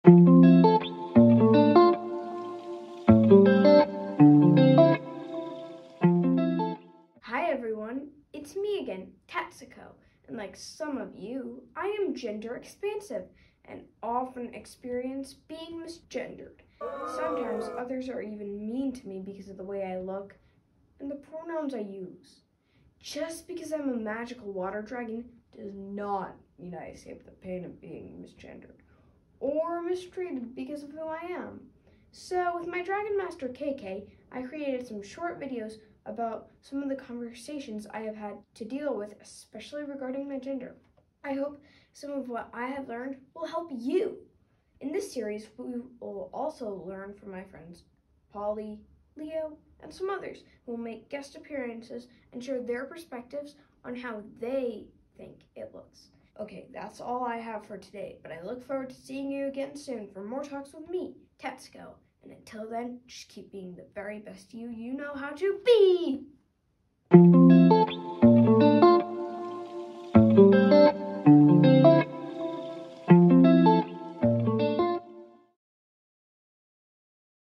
Introducing Talks with Tatsuko
In this video, Tatsuko reintroduces herself, discusses the pain of being misgendered, and shares about "Talks with Tatsuko", a series of short videos for trans/ gender expansive kids, that she and KK are creating.